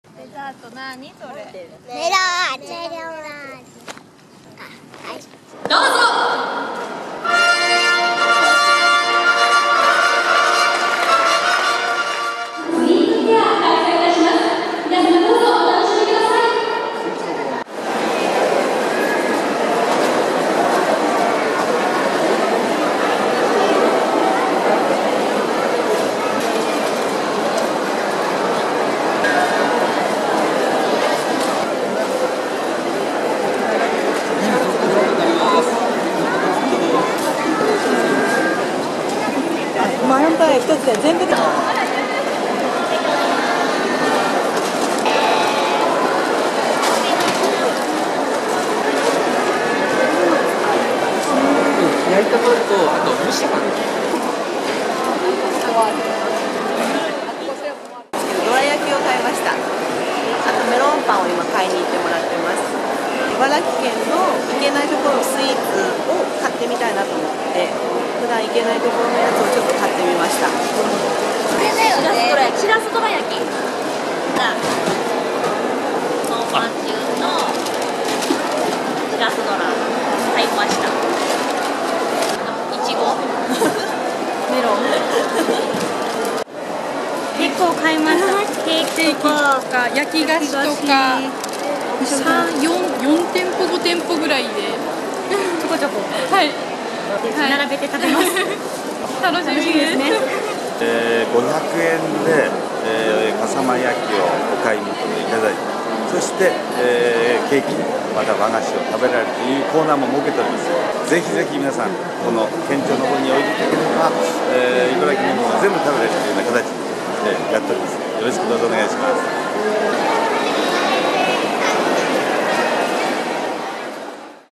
和洋菓子に家族にぎわう いばらきスイートフェア
本県の誇る和洋菓子などの有名店がそろう「いばらきスイートフェア」（同実行委員会主催）が24日、水戸市笠原町の県庁２階ホールを会場に始まり、大勢の家族連れらでにぎわった。
期間は日まで。

フェアは今年で２回目。この日は和洋菓子やパンなどブースと笠間焼のコーナーが出店された。
、の両日はこのほか、常陸牛やローズポークを使った和洋中の料理、笠間焼とスイーツを組み合わせたワンコイン（５００円）のセットが販売される。

東海村から来た親子連れは「普段は行けない店のスイーツを買ってみた」と話した。
実行委員長の根本高行県洋菓子協会長は「茨城の食材を使った菓子と料理を楽しんでほしい」と来場を呼び掛けていた。